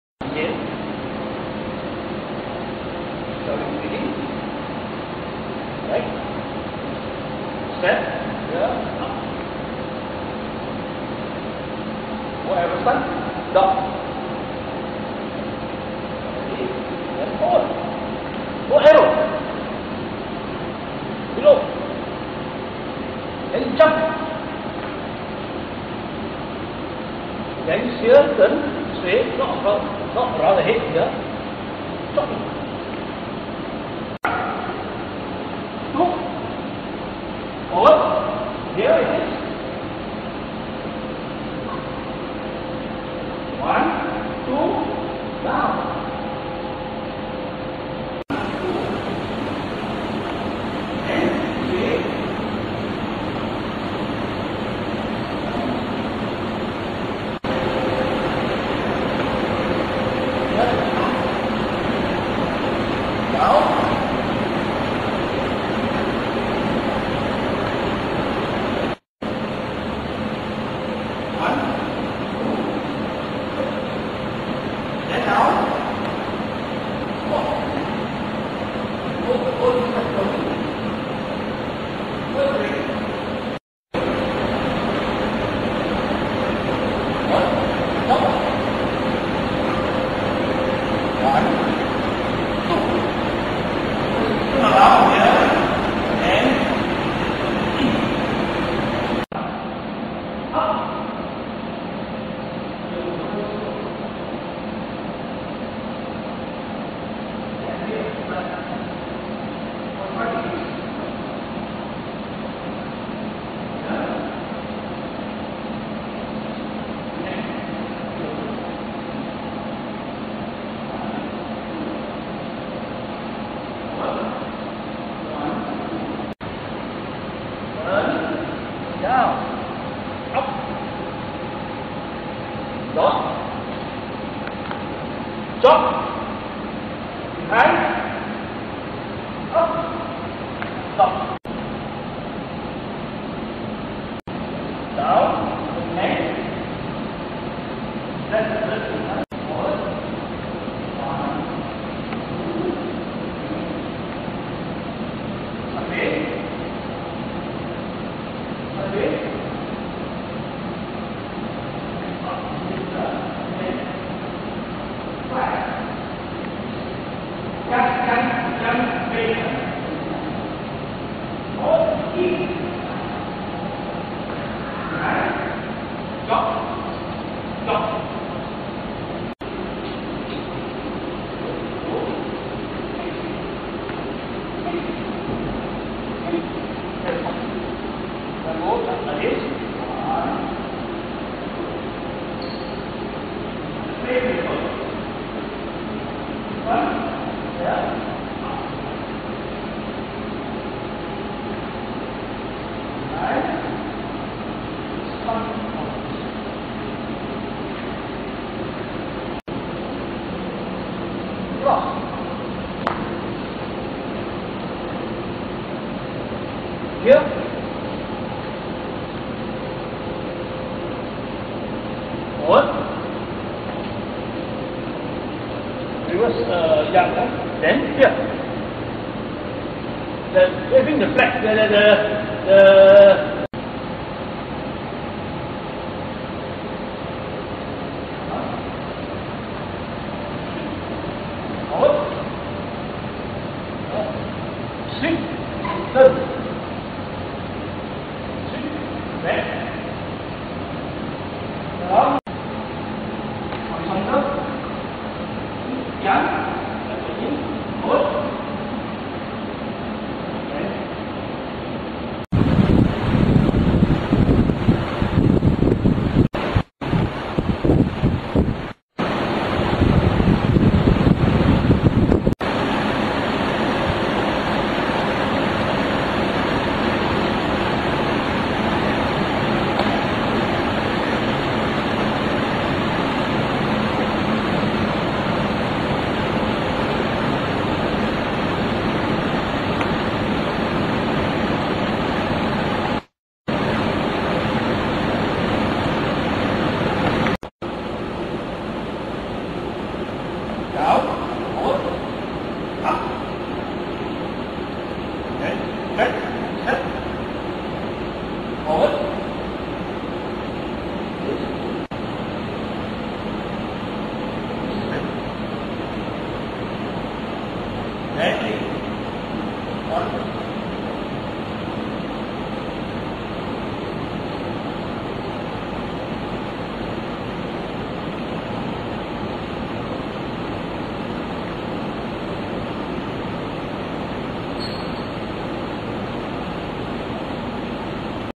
少林龍行劍 Shaolin Traveling Dragon Sword
The Traveling Dragon Sword at the Intensive Weapon Course of June 2006.